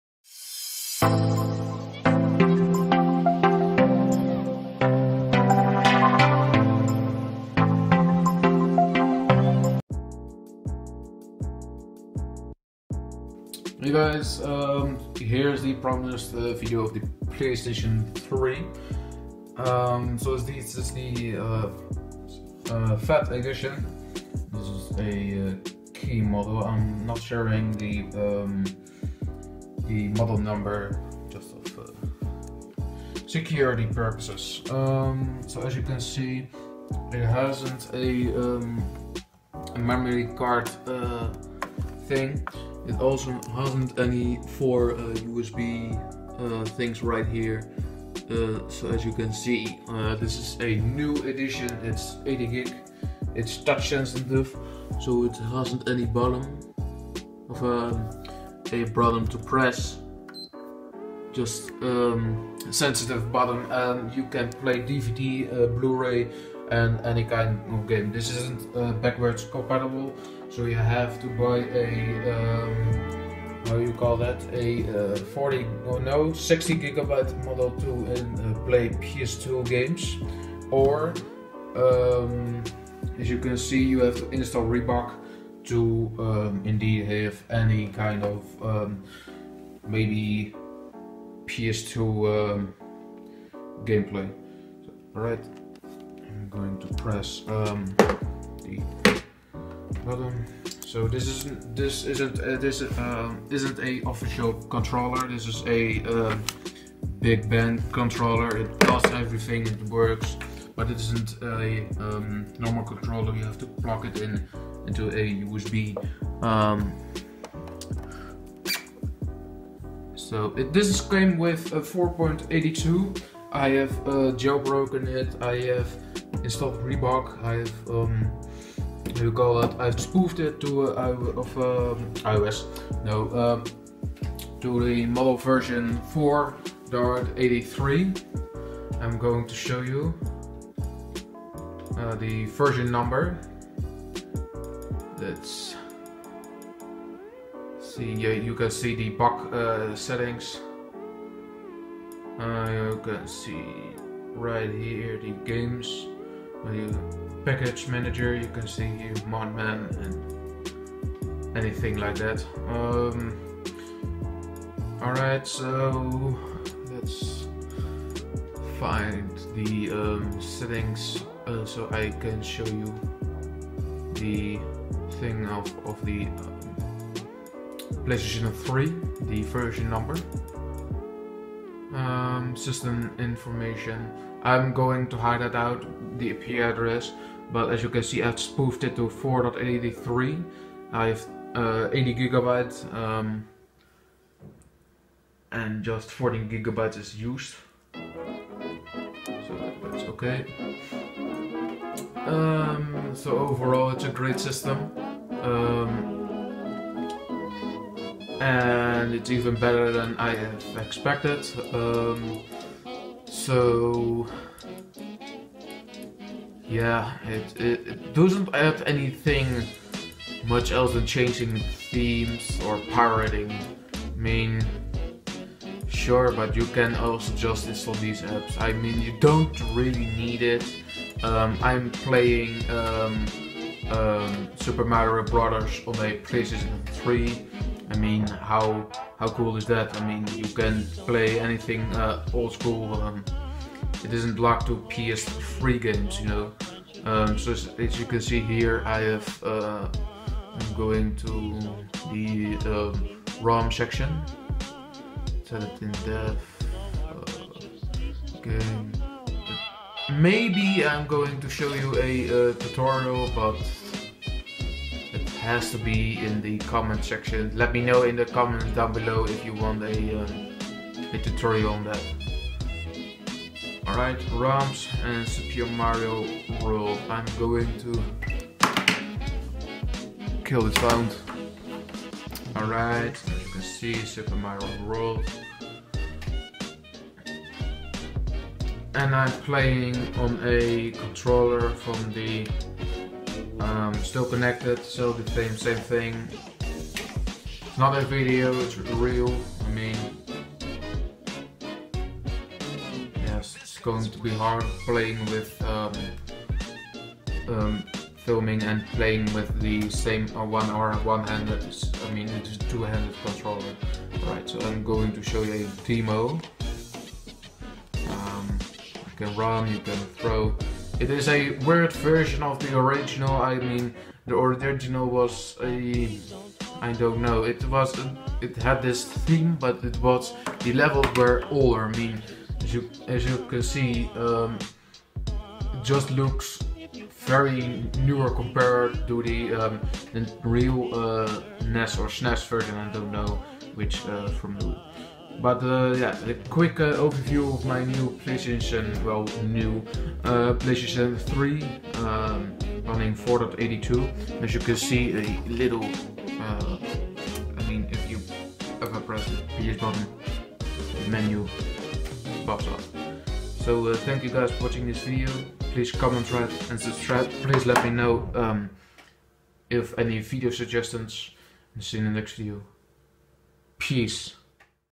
Phat PS3 80GB overview- no backwards compatibility (NEW VIDEO!)
Hi guys TechRager here!
I've bought a 'new' PS3 Phat, that i want to share with you to day.